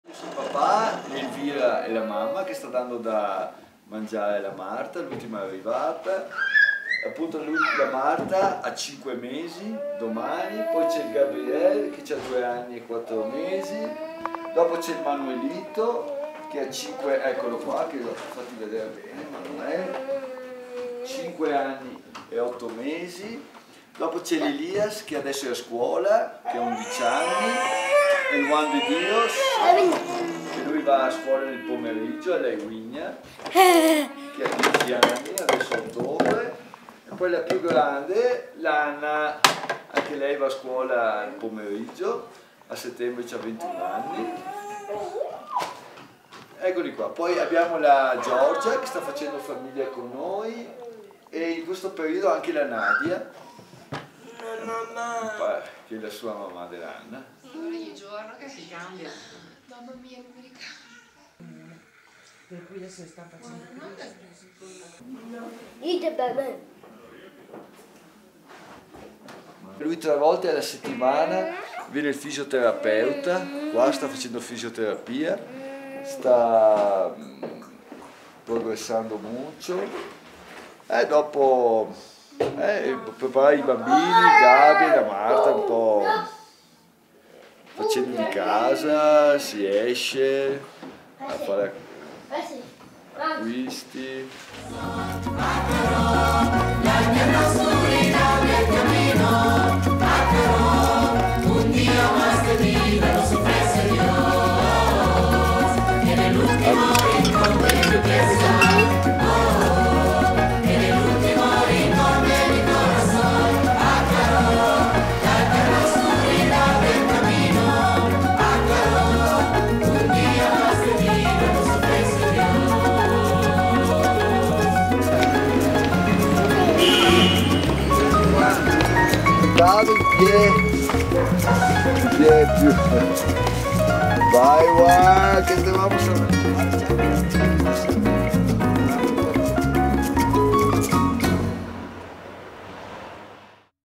Casa famiglia S.Francesco, la missione in Bolivia - Apg23
Sito Associazione Comunità Papa Giovanni XXIII:
La casa famiglia è il modello di accoglienza pensato e realizzato da Don Oreste Benzi. Questo modello che prevede la genitorialità e la multiutenza è presente in tutte le missioni della Comunità Papa Giovanni XXIII nel mondo. Questa è la casa famiglia "San Francesco" presente in Bolivia.